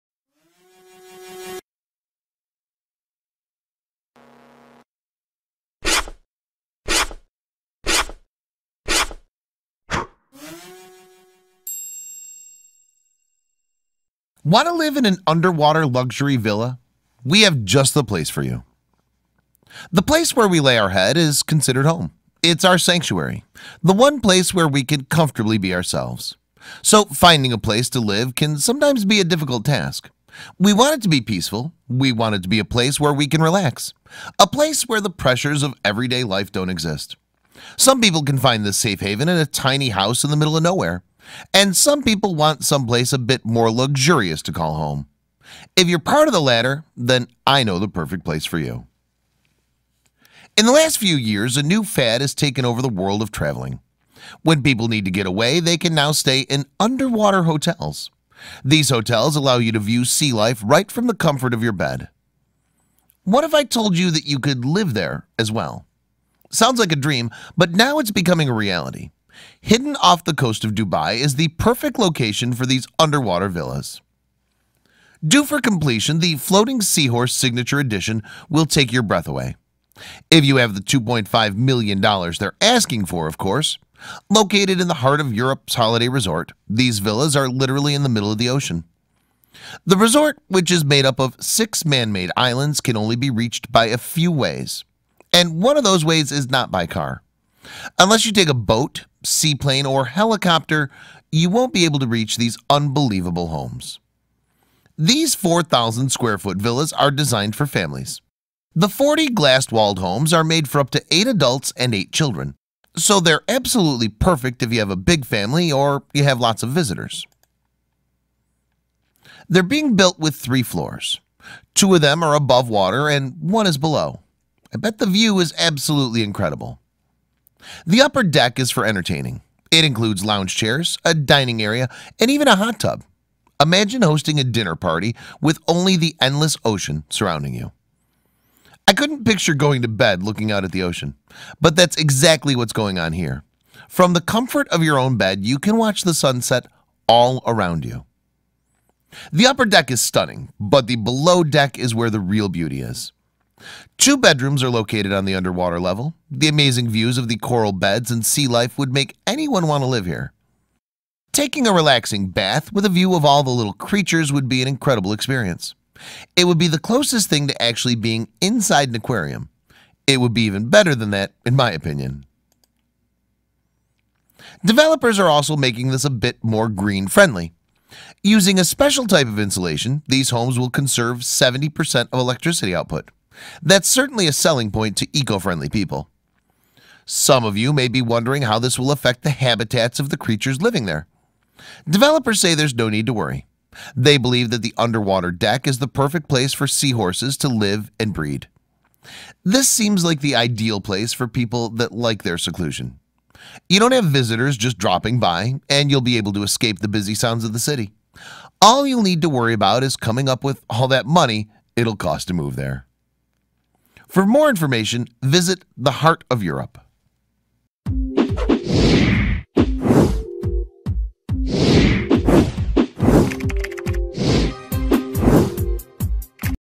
Want To Live In An Underwater Luxury Villa We Have Just The Place For You
The place where we lay our head is considered home. It's our sanctuary; the one place where we can comfortably be ourselves. So, finding a place to live can sometimes be a difficult task. We want it to be peaceful. We want it to be a place where we can relax. A place where the pressures of everyday life don't exist. Some people can find this safe haven in a tiny house in the middle of nowhere. And some people want some place a bit more luxurious to call home. If you're a part of the latter, then I know the perfect place for you!